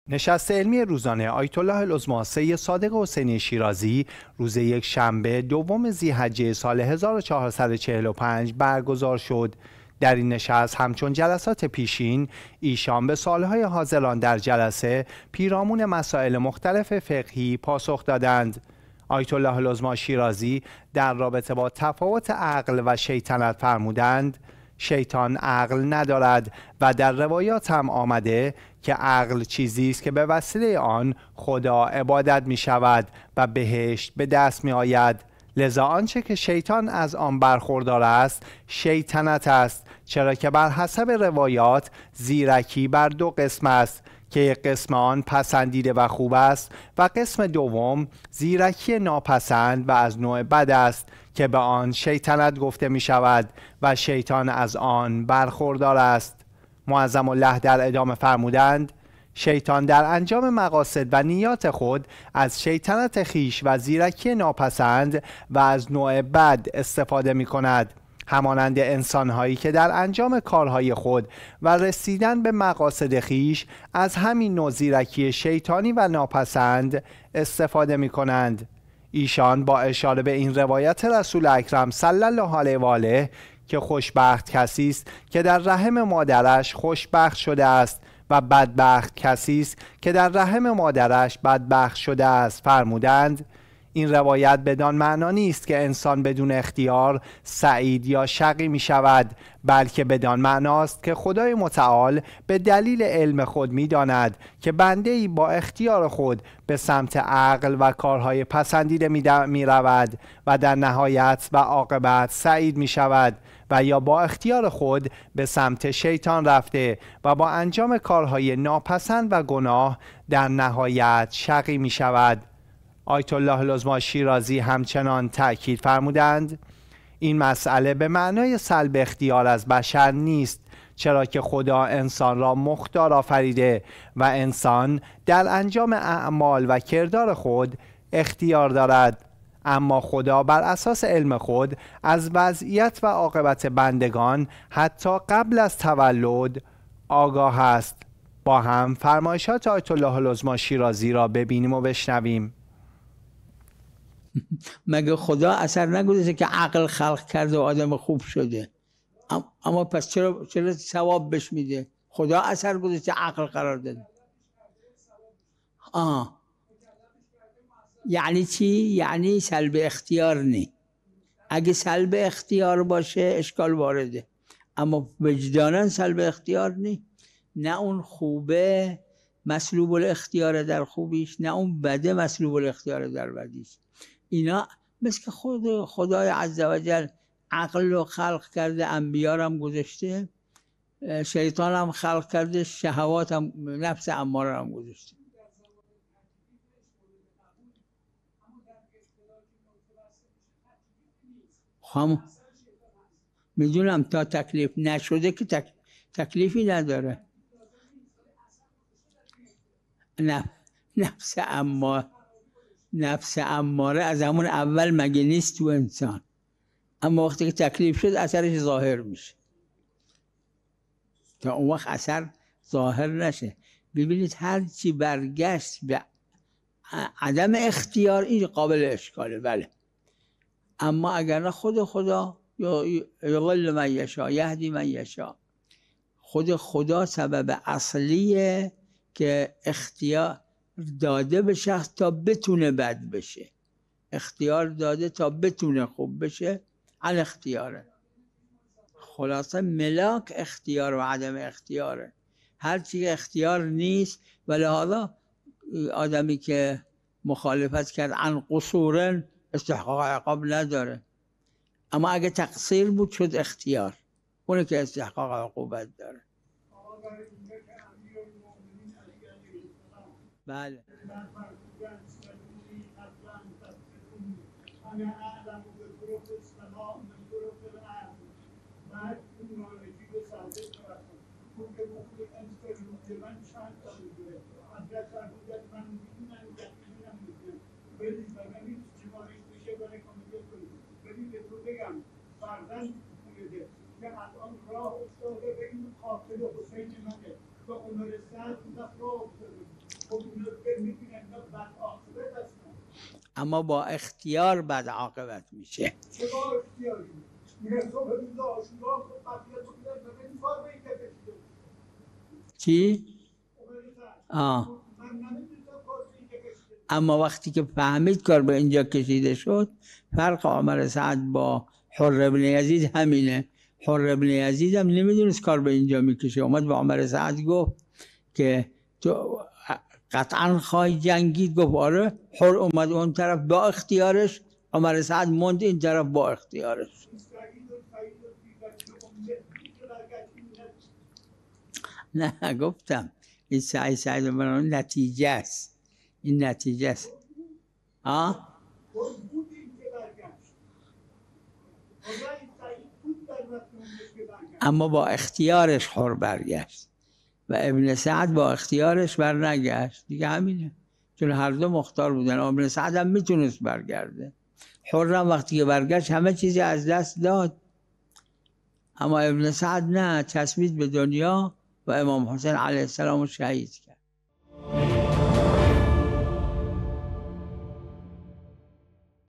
آیت الله العظمی شیرازی: شیطان عقل ندارد، و آنچه که از آن برخوردار است، زیرکی از نوع بد و شیطنت است
نشست علمی روزانه آیت الله العظمی سید صادق حسینی شیرازی، روز یکشنبه دوم ذی حجه سال ۱۴۴۵ برگزار شد، در این نشست هم‌چون جلسات پیشین، ایشان به سوال‌های حاضران در جلسه، پیرامون مسائل مختلف فقهی پاسخ دادند.

آیت الله العظمی شیرازی در رابطه با تفاوت عقل و شیطنت فرمودند: شیطان عقل ندارد، و در روایات هم آمده که: عقل چیزی است که به وسیله آن خدا عبادت می‌شود و بهشت به دست می‌آید، لذا آنچه که شیطان از آن برخوردار است، شیطنت است، چرا که بر حسب روایات زیرکی بر دو قسم است، که یک قسم آن پسندیده و خوب است، و قسم دوم، زیرکی ناپسند و از نوع بد است که به آن شیطنت گفته می‌شود، و شیطان از آن، برخوردار است.

معظم له در ادامه فرمودند: شیطان در انجام مقاصد و نیات خود، از شیطنت خویش و زیرکی ناپسند و از نوع بد استفاده می‌کند، همانند انسان‌هایی که در انجام کارهای خود و رسیدن به مقاصد خویش از همین نوع زیرکی شیطانی و ناپسند استفاده می‌کنند.

ایشان با اشاره به این روایت رسول اکرم صلی الله علیه و آله: خوش بخت، كسى است كه در رَحِم مادرش خوش بخت شده است و بدبخت، كسى است كه در رَحِم مادرش بدبخت شده است، فرمودند: این روایت بدان معنا نیست که انسان بدون اختیار، سعید یا شقی می‌شود، بلکه بدان معناست که خدای متعال به دلیل علم خود، می‌داند که بنده ای با اختیار خود به سمت عقل و کارهای پسندیده می‌رود و در نهایت و عاقبت سعید می‌شود، و یا با اختیار خود به سمت شیطان رفته و با انجام کارهای ناپسند و گناه، در نهایت شقی می‌شود.

آیت الله العظمی شیرازی هم چنین تاکید فرمودند: این مساله به معنای سلب اختیار از بشر نیست، چرا که خدا انسان را مُختار آفریده و انسان در انجام اعمال و کردار خود اختیار دارد، اما خدا بر اساس علم خود، از وضعیت و عاقبت بندگان حتی قبل از تولد آگاه است.